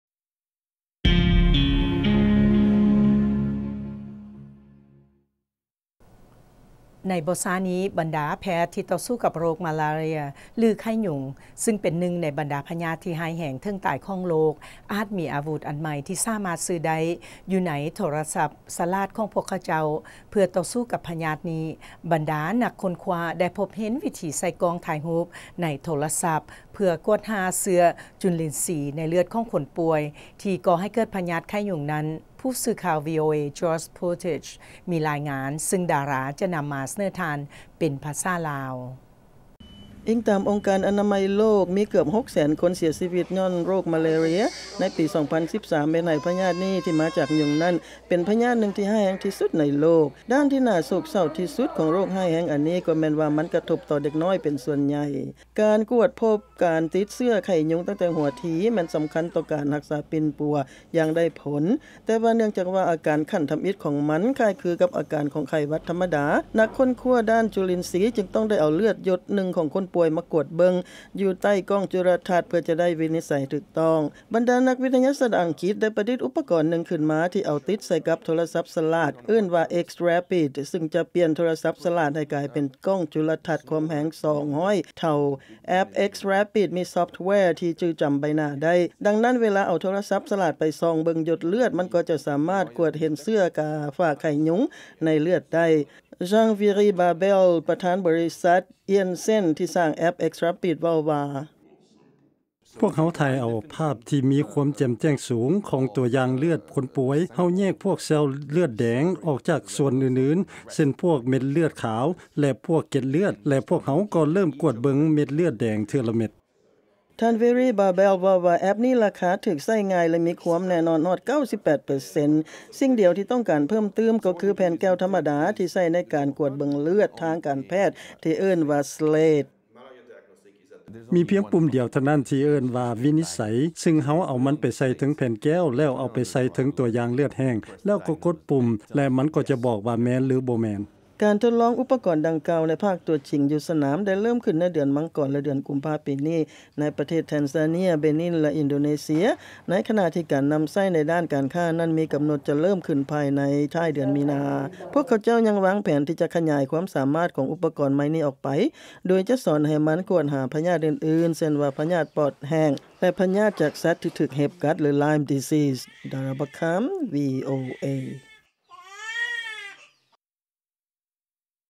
ໂທລະສັບສະຫລາດ ໃຊ້ເປັນ ເຄື່ອງມືວິນິດໄສ ໂຣກມາລາເຣຍໄດ້
ໃນ​ບໍ່​ຊ້າ​ນີ້ ບັນ​ດາ​ແພດອາດມີ​ອາວຸດ​ອັນ​ໃໝ່ ​ທີ່​ສາມາດ​ຊື້​ໄດ້
ຢູ່​ໃນໂທລະສັບ​ສະຫລາດ ​ຂອງພວກ​ເຂົາ​ເຈົ້າ​ ​ເພື່ອ​ຕໍ່ສູ້​ກັບ​
ໂຣກມາລາ​ເຣຍນັ້ນ. ນັກ​ຄົ້ນຄວ້າໄດ້​ພົບເຫັນ​ວິທີ​ໃຊ້​ກ້ອງ​
ຖ່າຍຮູບ​ໃນໂທລະສັບ ​ເພື່ອ​ກວດ​ຫາ ຈຸ​ລິນ​ຊີ ໃນ​ເລືອດ​
ຂອງຄົນ​ປ່ວຍ ທີ່​ກໍ່​ໃຫ້​ເກີດ​ພະຍາດນັ້ນ.